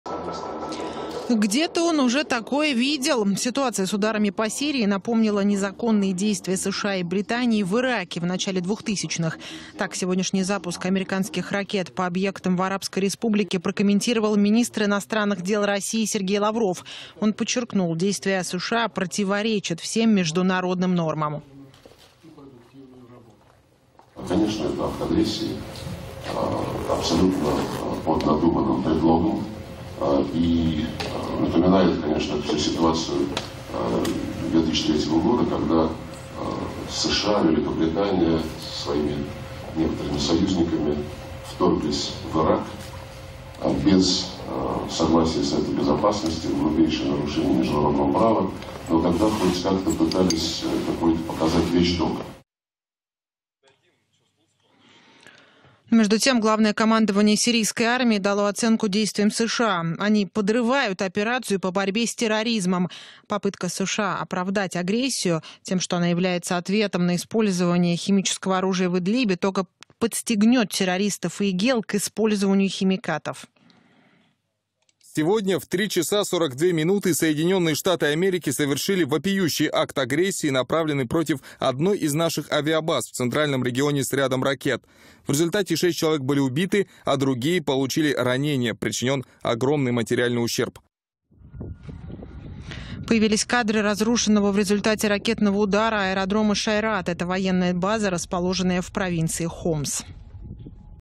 Россия приостанавливает действие Меморандума об избежании инцидентов с США в Сирии
Глава МИД России Лавров сравнил удар США по Сирии с вторжением в Ирак в 2003 году